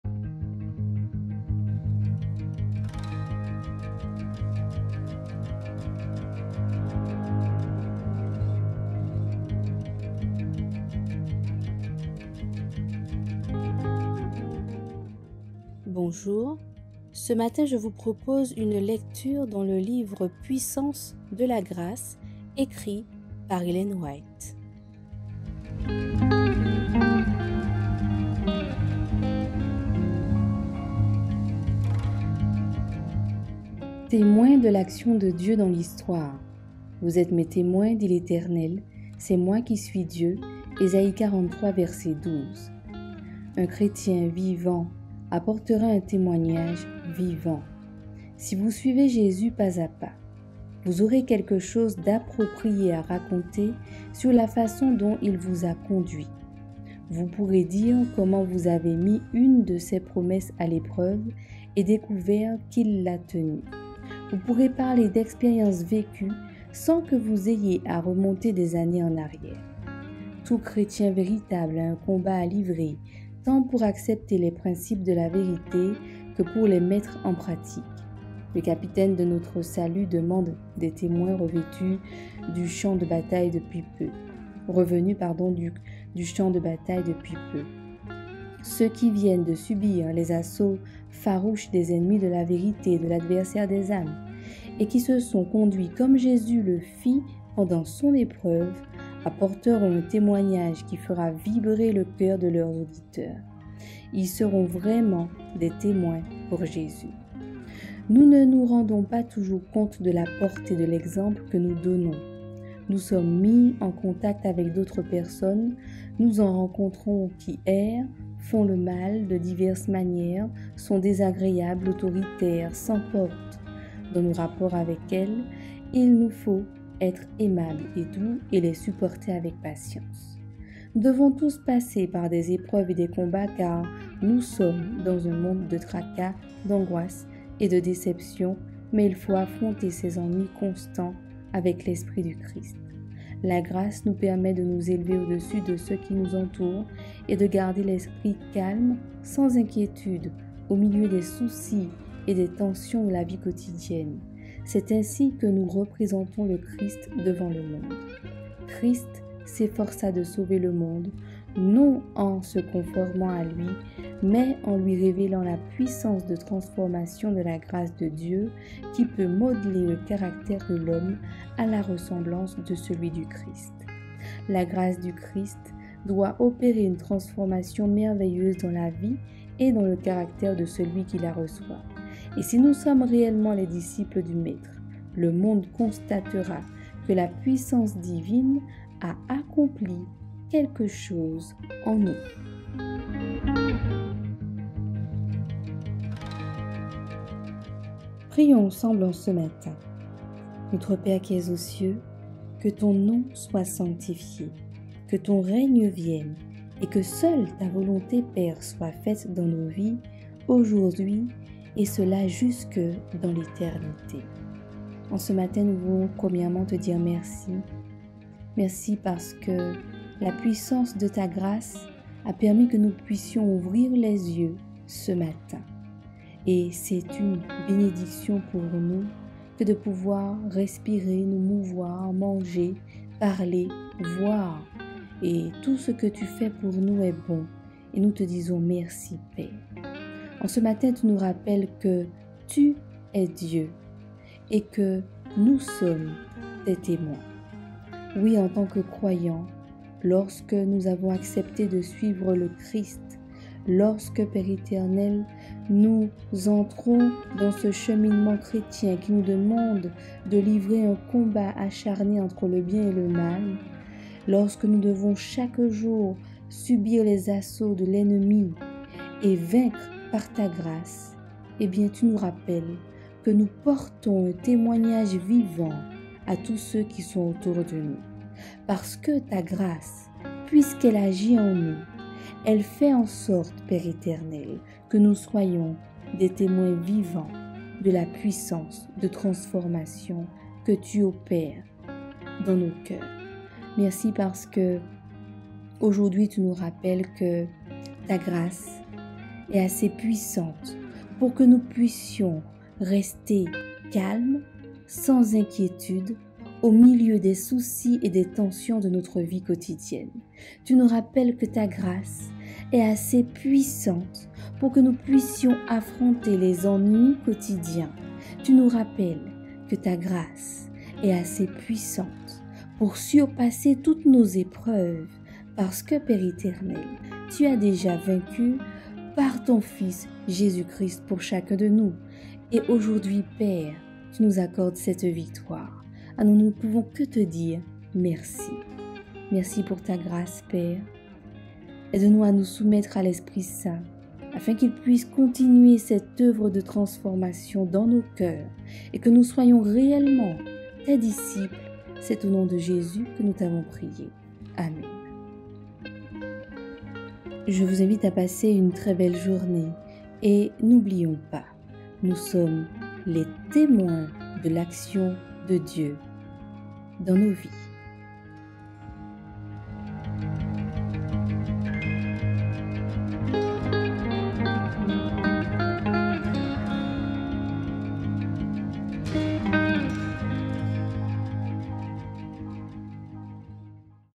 MEDITATION MATINALE: TEMOINS DE L'ACTION DE DIEU DANS L'HISTOIRE
MEDITATION MATINALE 7 AOÛT

Vous êtes mes témoins, dit l'Eternel, c'est moi qui suis Dieu.
Ésaïe 43:12.